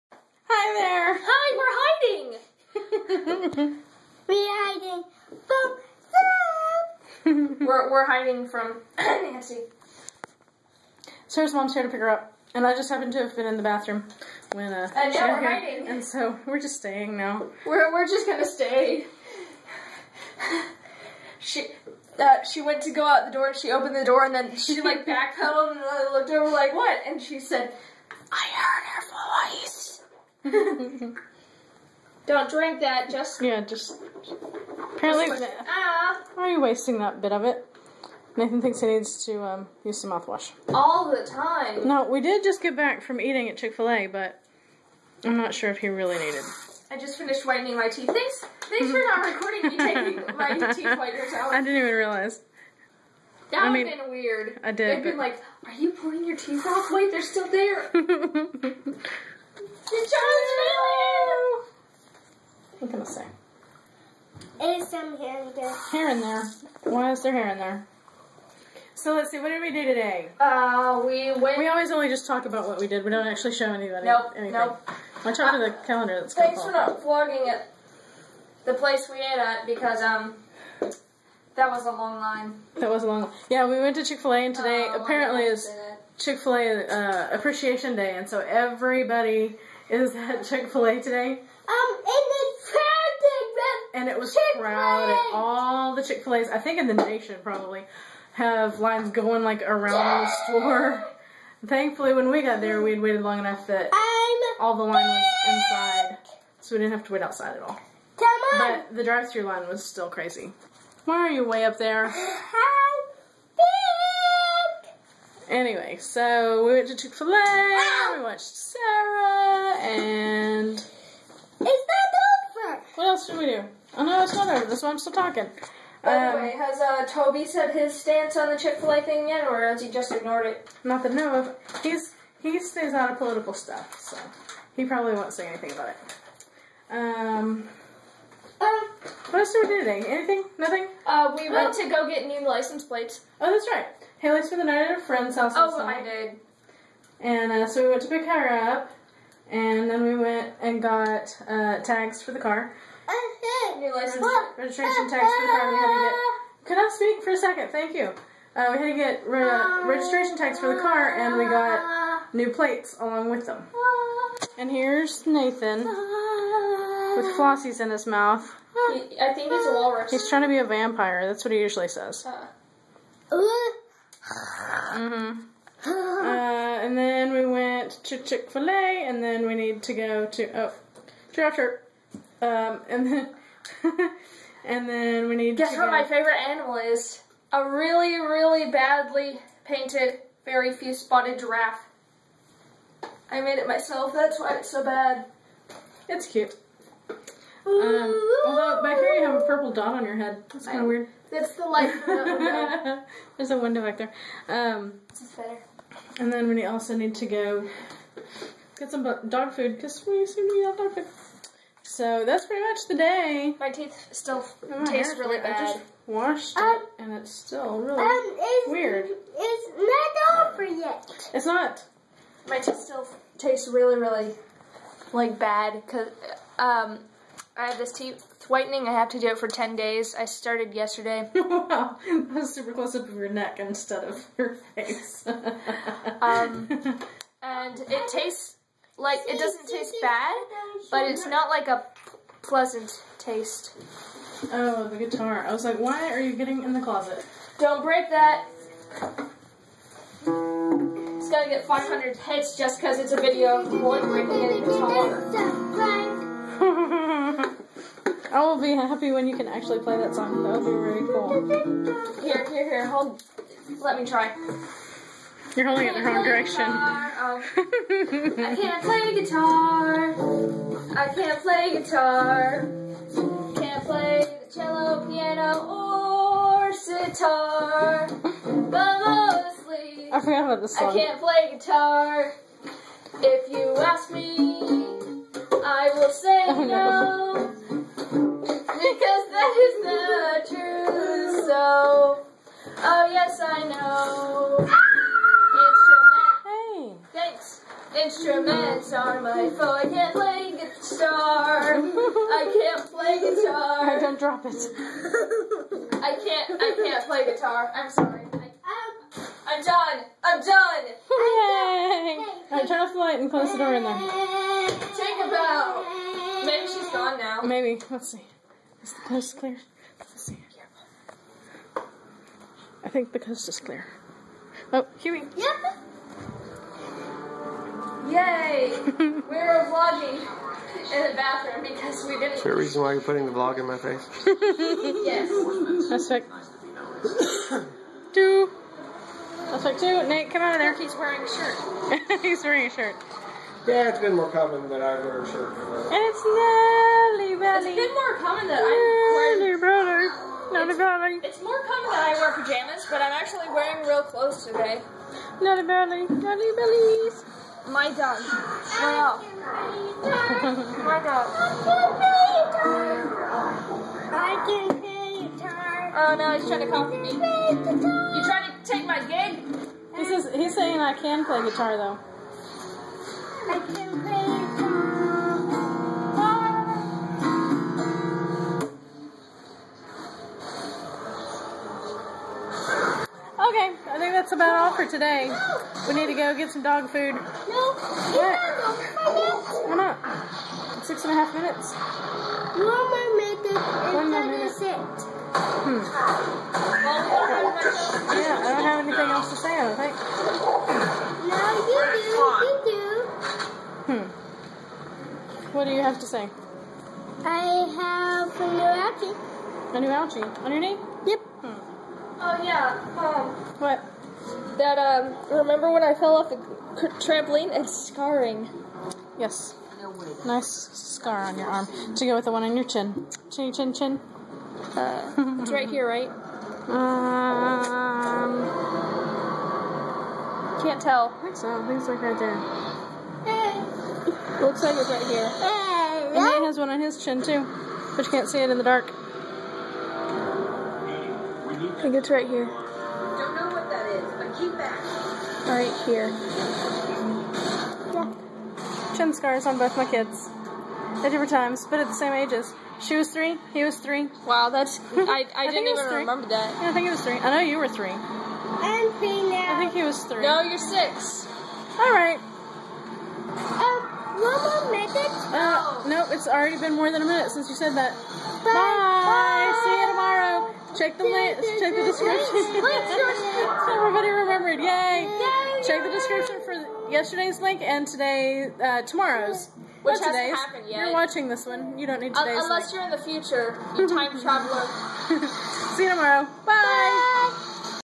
Everybody HIDE!!
Day 34 -- It's Chick-fil-A Appreciation Day! And Hayley sings a song!